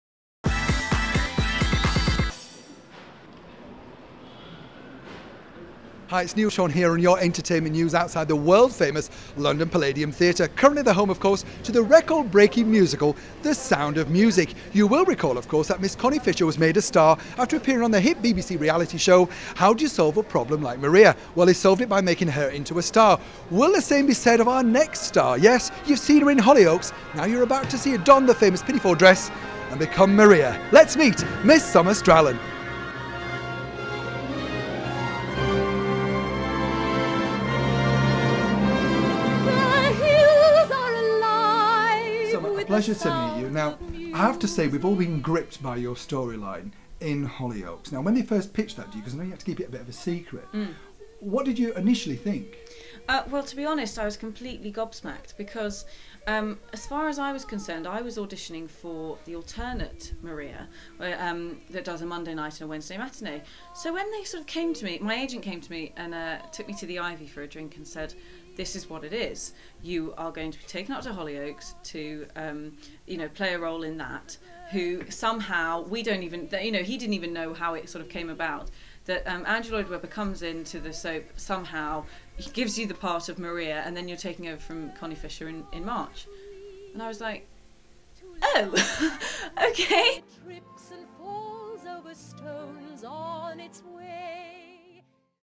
Neil Sean meets Summer Strallen
In this exclusive interview filmed backstage at the London Palladium, Neil chats to Summer Strallen about her role in Hollyoaks and moving on to star as 'Maria' in The Sound of Music.